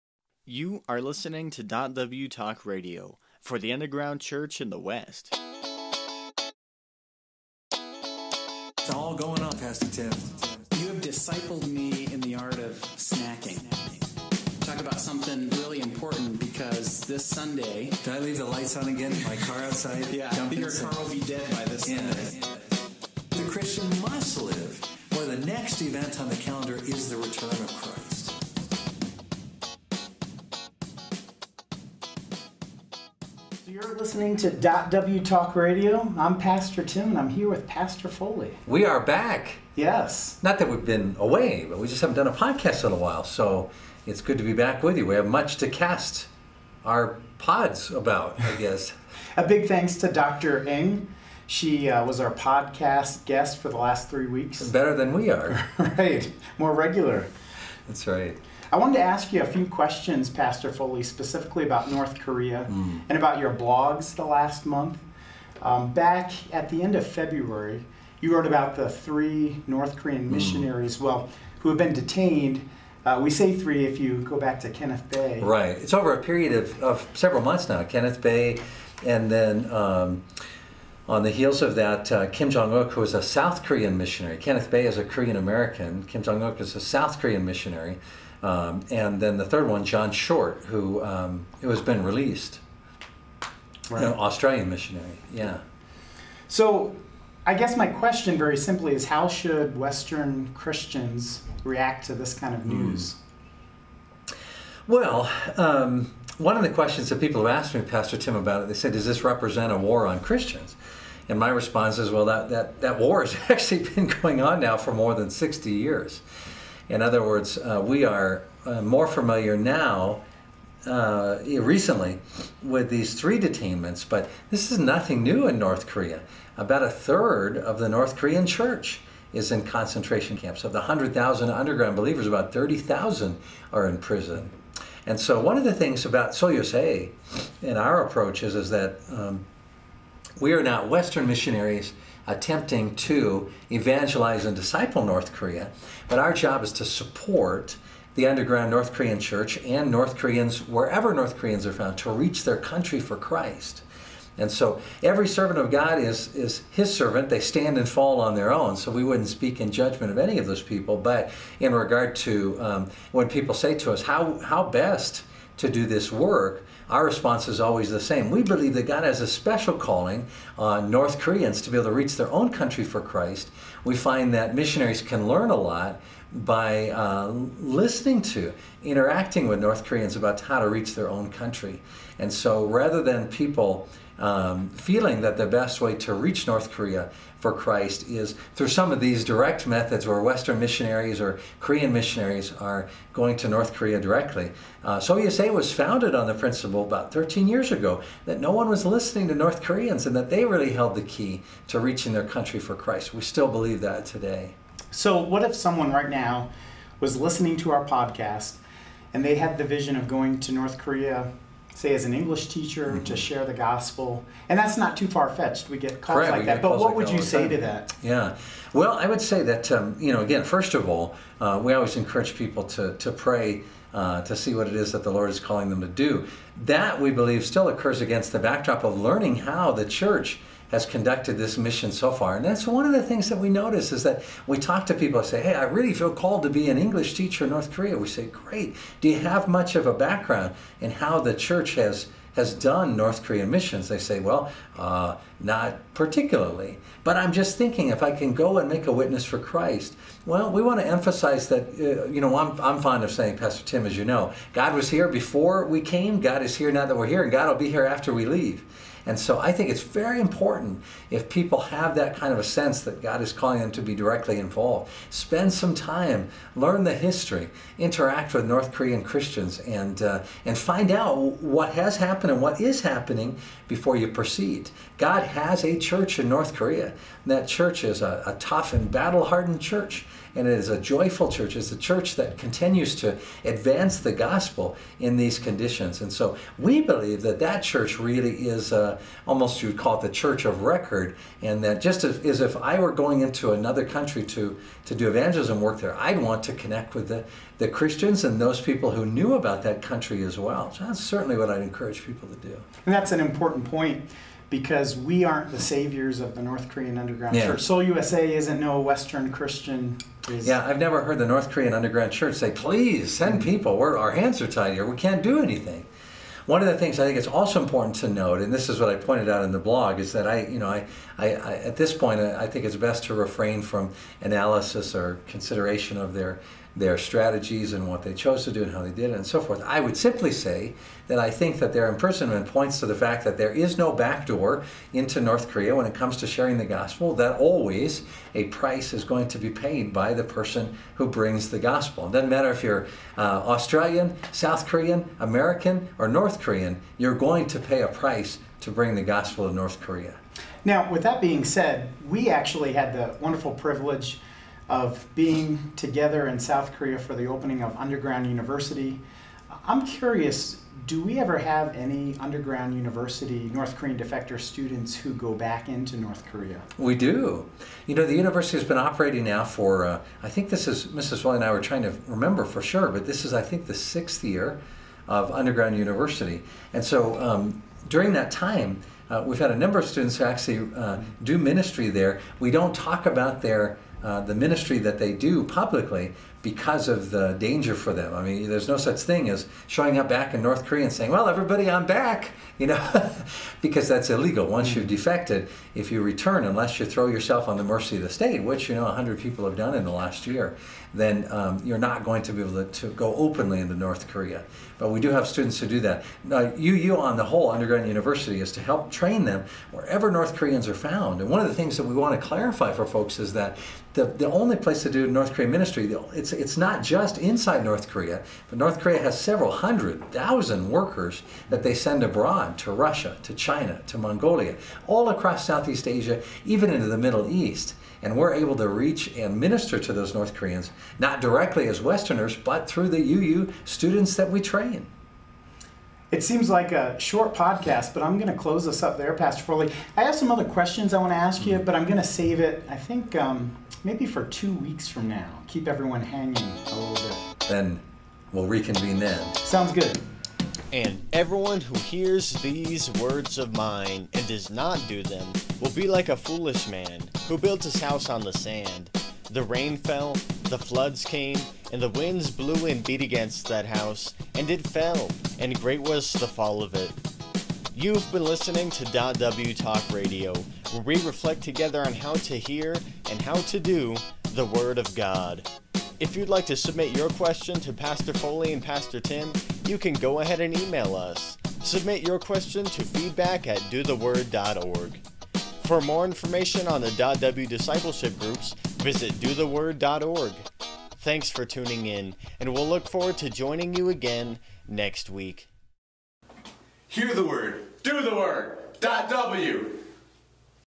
North Korean Detainees Podcast
Pastor Tim asks Pastor Foley about North Korean missionaries, Kenneth Bae, Kim Jong Uk and Jonathan Short.  They also give advice to individuals who may be feeling a calling to do mission work in North Korea.